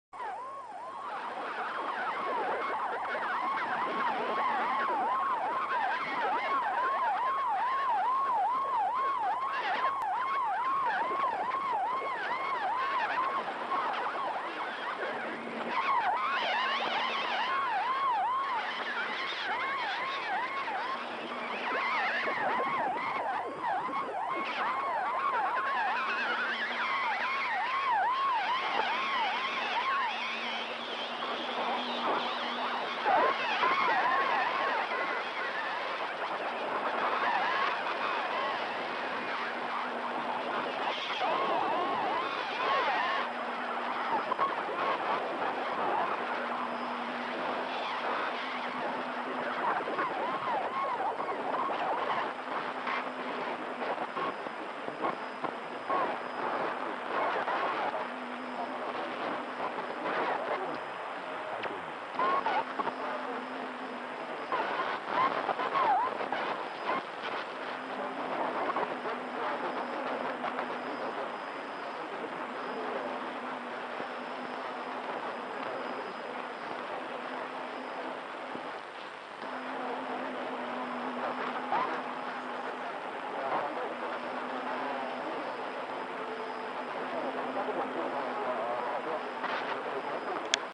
Just a little bit freaky. 9715kHz alarm noise goes bonkers.
This thing is haunting me on the shortwave bands.

This time it's only 15kHz up from where it was a few days ago, and I'd only been tuning around the bands for a few minutes when it came up.

At the start of the clip, there seems to be more than one alarm and other phenomena going on.

Towards the end of the clip, it seems to break up as if it's having difficulties.

Some weird stuff...

Incidentally, this frequency has some history - 9715kHz was one frequency used by US forces during the initial stages of the Iraq War for 'Information Radio' broadcast over Iraq.